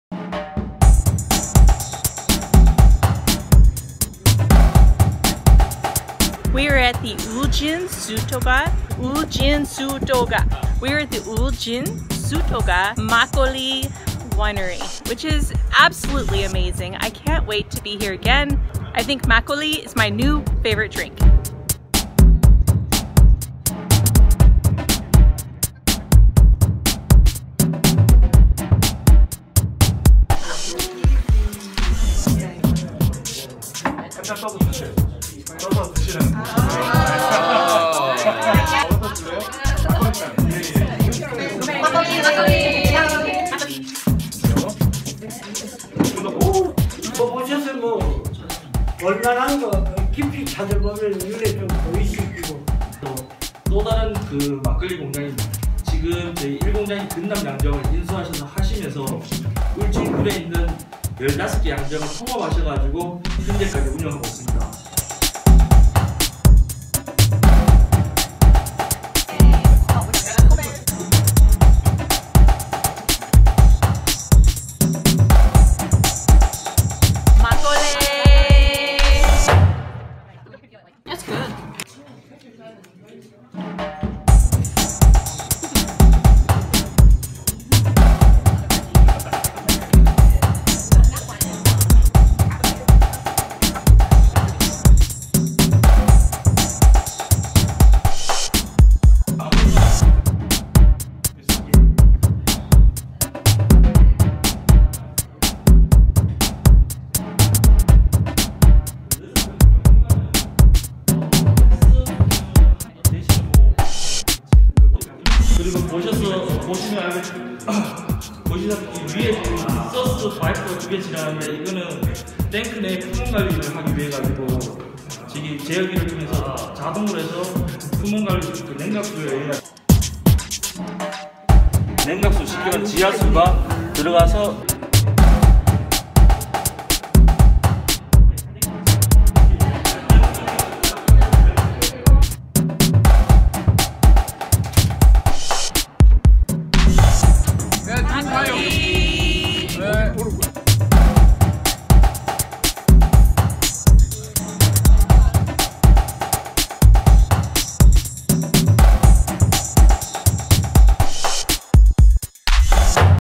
Travel Korea: Makgeolli Wine Tour at Uljin Suldoga @Amy Moncure
Travel Korea: Uljin Suldoga 울진군   술도가 Makgeolli Wine Tour

The Uljin Suldoga Makgeolli Winery has been family owned since 1920.  Currently the father and son pair, Hong Soon-young 홍순영 and Hong Soon-young 홍순영 work together to create an amazing makgeolli.
     For those of you new to makgeolli, it is a traditional Korean rice wine.  Makgeolli used to be known as farmer's wine.  However, lately it is being enjoyed by the younger generation and is becoming more mainstream.
     Being in Uljin, the famous crab region of Korea, the seafood provided at the Uljin Suldoga 울진군   술도가 wine tour was absolutely amazing!!!

A local Uljin article describing Uljin Suldoga as a local roots establishment